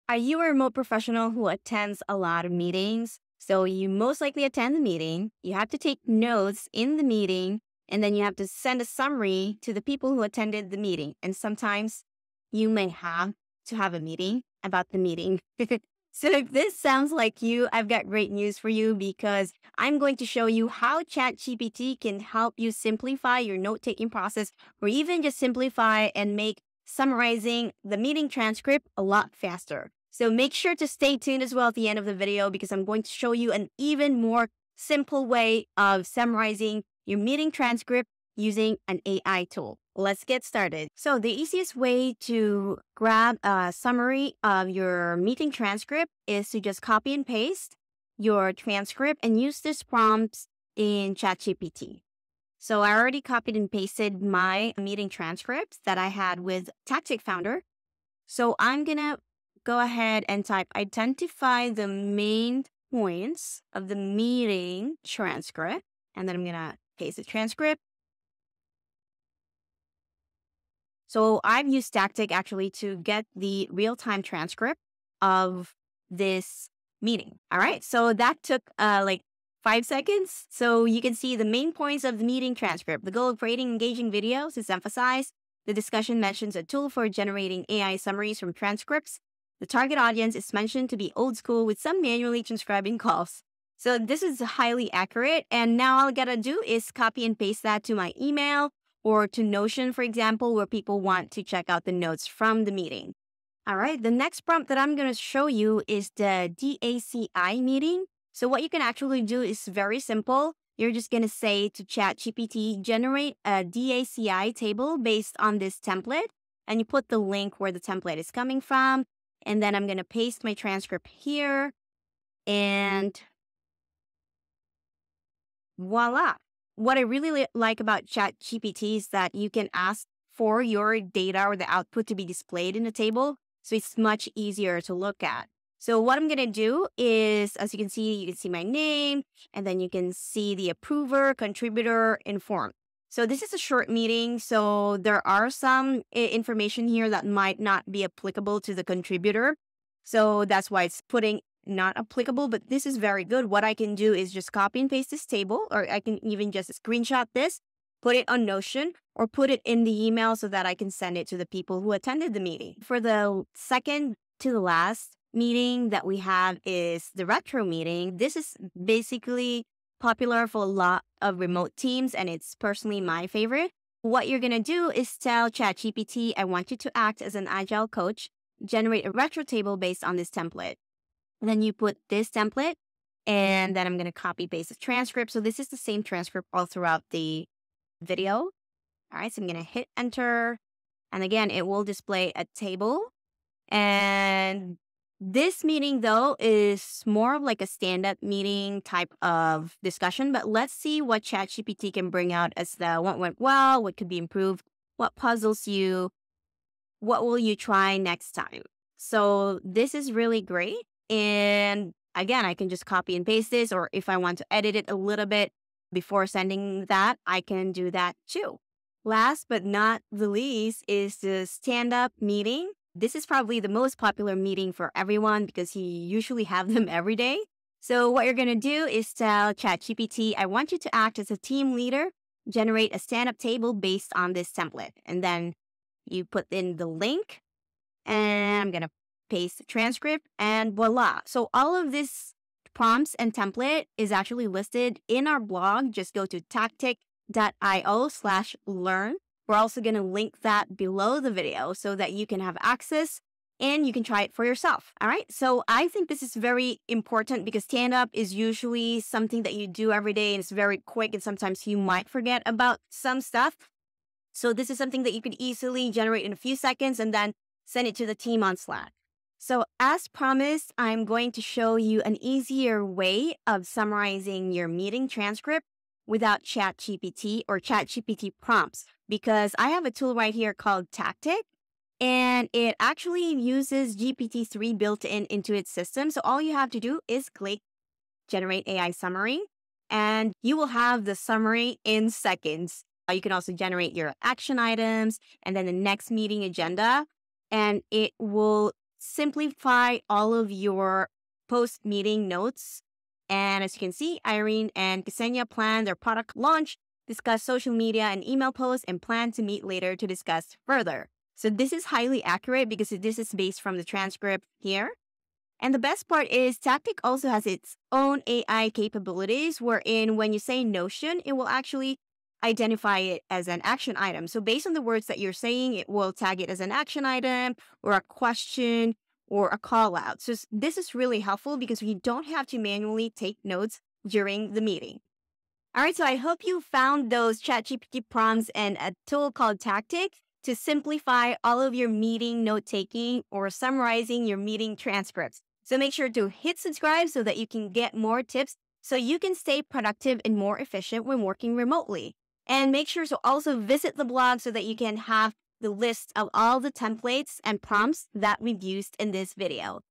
Use These 5 ChatGPT Prompts to Streamline Your Meeting Note-Taking Process | Step-by-Step Guide
Discover how to simplify your meeting note-taking process with ChatGPT and Tactiq! In this video, I'll show you how to use ChatGPT-3 to generate accurate and concise meeting summaries that save you time and effort.

Professionals and remote teams have been leveraging the power of ChatGPT for a wide range of tasks, from content creation to coding. But did you know that ChatGPT can also revolutionize the way you handle meeting notes? In this video, I'll walk you through various ChatGPT prompts that will help you create AI-powered meeting summaries, DACI tables, fill retro templates, and run stand-up meetings with ChatGPT.

DACI (decision-making framework that stands for Driver, Approver, Contributor, and Informed)

After watching this video, you'll be equipped with the knowledge to create comprehensive meeting summaries and action items in just a few seconds.

So if you're tired of manually transcribing meeting notes and struggling to summarize key points, give ChatGPT and Tactiq a go and use them as your AI-powered meeting assistants! In this video, you'll learn how to use ChatGPT and Tactiq to generate meeting summaries that keep everyone on the same page and boost team productivity.

With ChatGPT and Tactiq, you can effortlessly generate high-quality meeting summaries that capture the essence of your discussions. Simply input your meeting transcript and let ChatGPT and Tactiq do the rest, providing you with a well-organized summary that's ready to share. And as a bonus, I'll show you how Tactiq's built-in GPT-3 and GPT-4 capabilities can further enhance your meeting notes.

Now you know how to simplify your meeting note-taking process with AI. Good luck!